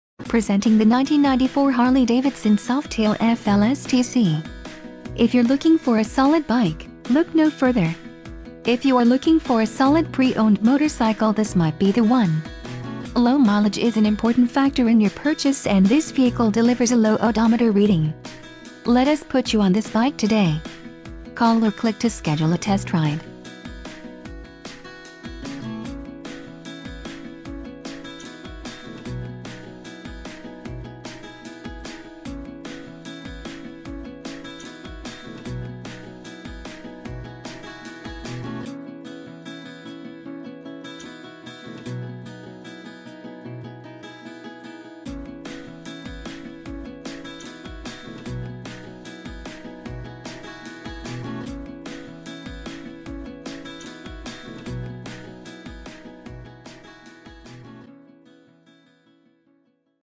1994 Harley-Davidson FLSTC Heritage Softail Classic - Dre...
YOU ARE LOOKING AT A 1994 HARLEY DAVIDSON HERITAGE SOFTAIL CLASSIC (FLSTC) WITH 40,938 MILES ON IT. IT IS VIVID BLACK IN COLOR AND POWERED BY A 1340CC (80") CARBURETED ENGINE AND 5 SPEED TRANSMISSION. THE BIKE COMES WITH 1 KEY. THERE ARE SOME EXTRAS ON THE BIKE WHICH INCLUDE CHROME EXHAUST, HI-FLO AIR CLEANER, CHROME FRONT END, BRAKE/SHIFTER PEDAL, CARLINI BARS, DASH PANEL, GRIPS, SWITCHES, SWITCH HOUSINGS, BRAKE/CLUTCH LEVERS, BRAKE/CLUTCH PERCH, MIRRORS, GAS CAP, PIVOT BOLT COVER, BELT SPROCKET, BELT COVER, AND BADLANDER SEAT. THE BIKE HAS SOME SMALL, MINOR IMPRESSIONS AND TOUCHUPS ON THE FRONT FENDER AND TANK AND TOUCHUPS AND SCUFFS ON THE REAR FENDER. THE CHROME AND ALUMINUM ARE IN GOOD CONDITION WITH THE EXCEPTION OF SCUFFS ON THE EXHAUST, SOME DISCOLORATION ON ENGINE, AND SOME FLAKING ON THE WHEELS AND HAND CONTROLS.